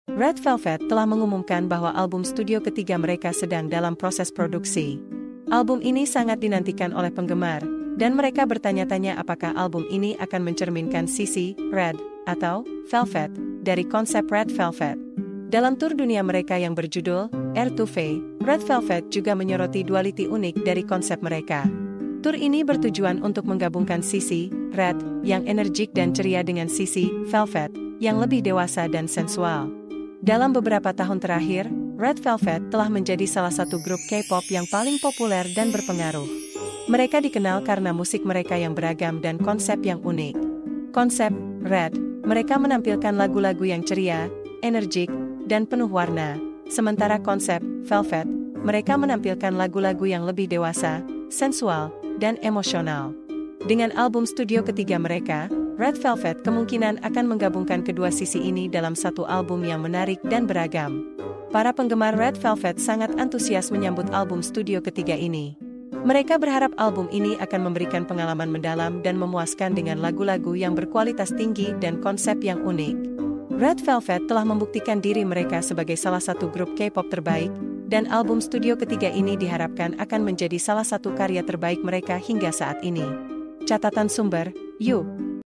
**Red Velvet Siap Rilis Album Studio Ketiga dengan Konsep yang Menarik dan Beragam**
Red Velvet telah mengumumkan bahwa album studio ketiga mereka sedang dalam proses produksi. Album ini sangat dinantikan oleh penggemar, dan mereka bertanya-tanya apakah album ini akan mencerminkan sisi "red" atau "velvet" dari konsep Red Velvet. Dalam tur dunia mereka yang berjudul "R to V," Red Velvet juga menyoroti duality unik dari konsep mereka. Tur ini bertujuan untuk menggabungkan sisi "red" yang enerjik dan ceria dengan sisi "velvet" yang lebih dewasa dan sensual .

Dalam beberapa tahun terakhir, Red Velvet telah menjadi salah satu grup K-pop yang paling populer dan berpengaruh. Mereka dikenal karena musik mereka yang beragam dan konsep yang unik. Konsep "red" mereka menampilkan lagu-lagu yang ceria, enerjik, dan penuh warna, sementara konsep "velvet" mereka menampilkan lagu-lagu yang lebih dewasa, sensual, dan emosional. Dengan album studio ketiga mereka, Red Velvet kemungkinan akan menggabungkan kedua sisi ini dalam satu album yang menarik dan beragam .

Para penggemar Red Velvet sangat antusias menyambut album studio ketiga ini. Mereka berharap album ini akan memberikan pengalaman mendalam dan memuaskan dengan lagu-lagu yang berkualitas tinggi dan konsep yang unik. Red Velvet telah membuktikan diri mereka sebagai salah satu grup K-pop terbaik, dan album studio ketiga ini diharapkan akan menjadi salah satu karya terbaik mereka hingga saat ini .

**Catatan:** Sumber: You.com